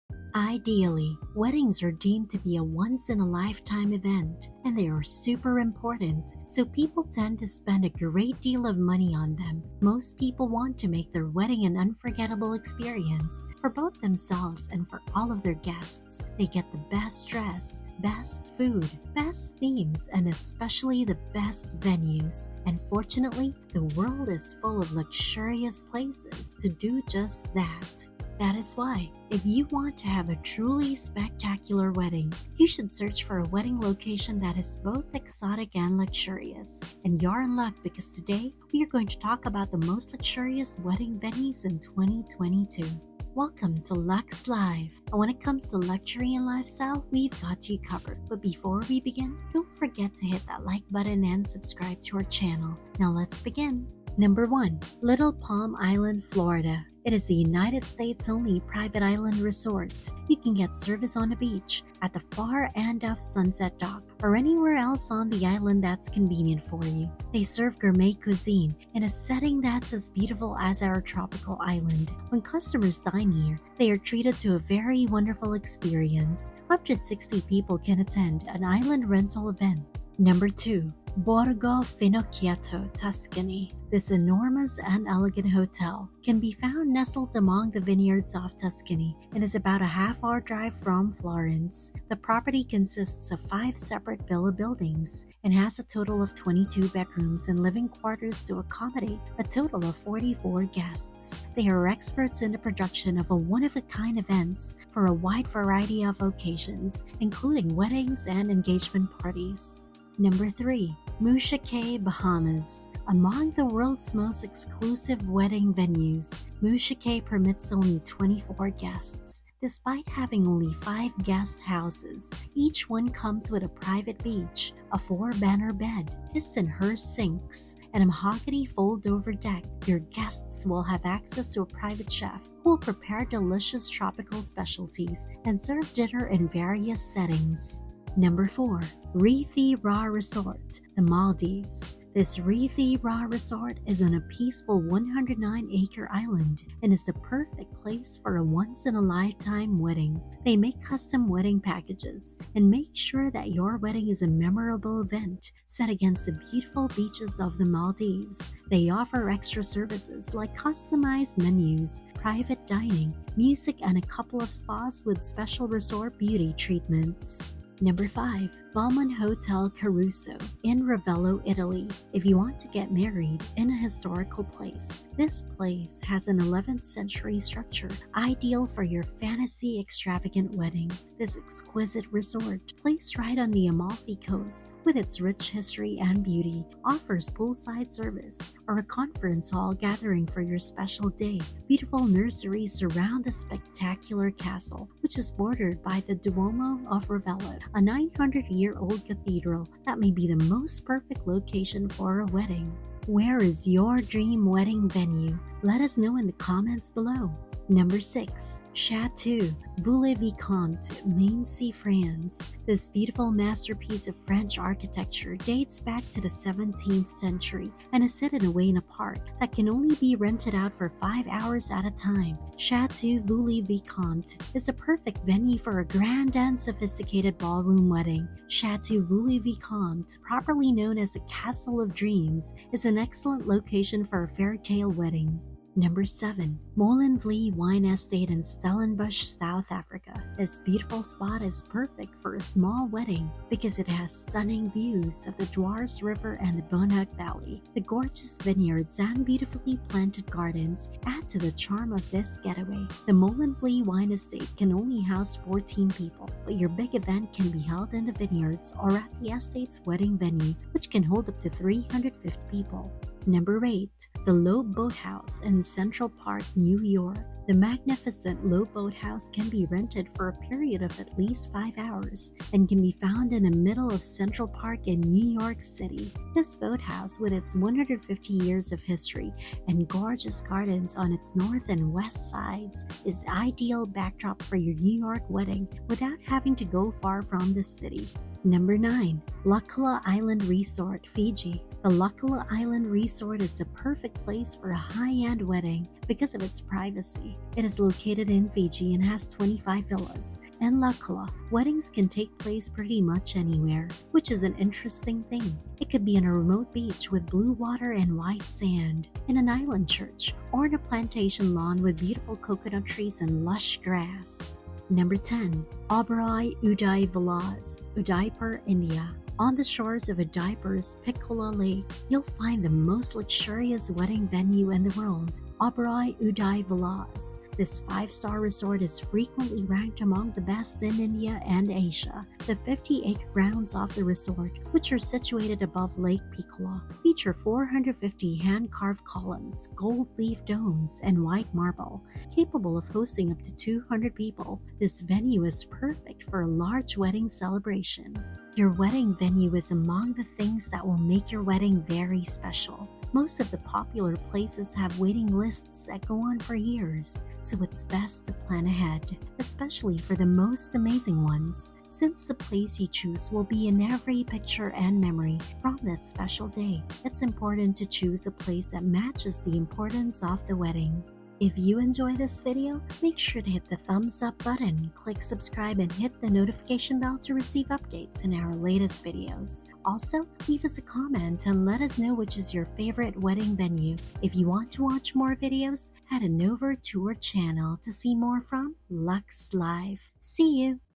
10 Top Wedding Venues in 2022
The wedding proposal was done, the date has been arranged, and the ring has been put on your finger. It's time to choose the venue for the most meaningful day of your life! So you may want to look for a unique, luxurious place for your wedding. We've curated a list of our 10 top wedding venues in 2022.

Welcome to LuxLife.  On this channel, we share with you Luxury People, Luxury items, Luxury places, and Luxury tips and info for your life.